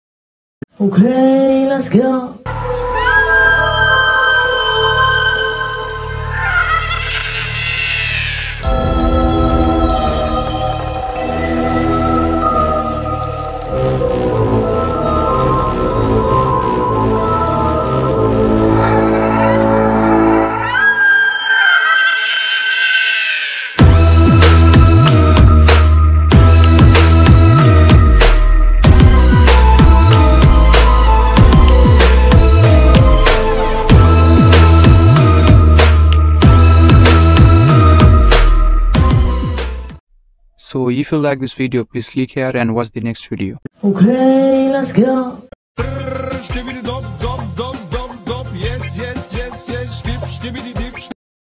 What? a large rock falls on the car and breaks the car on google earth
Hey What's up guys! In this video I am going to show you a large rock falls on the car and breaks the car on Google Earth! It's so weird.

Guys! If you like my videos, you can support me using the "Sponsor" and "Super Thanks" buttons.